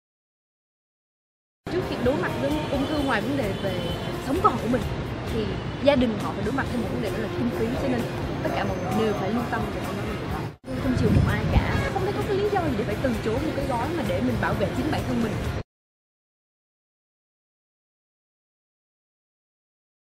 Tóc Tiên chia sẻ về bảo hiểm ung thư FWD
FWD Bảo hiểm hỗ trợ viện phí - Câu hỏi thường gặp

Quyền lợi bảo hiểm viện phí được chi trả khi nào?
-Khách hàng điều trị nội trú (nằm viện điều trị qua đêm tại Bệnh viện).
-Khách hàng điều trị trong ngày có phẫu thuật tại Bệnh viện nhưng không nằm viện qua đêm.